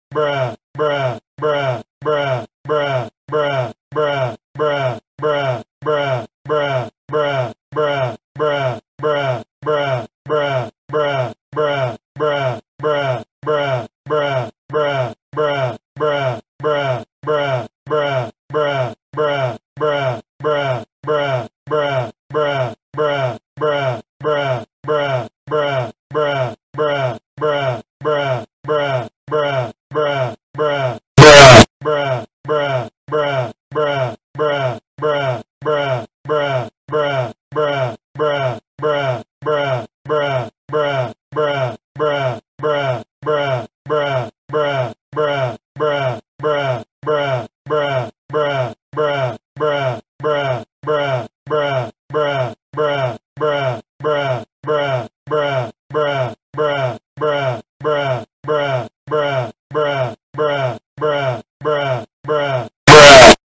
The Bruh Sound Effect but There's 100
2 of them are a little louder then the rest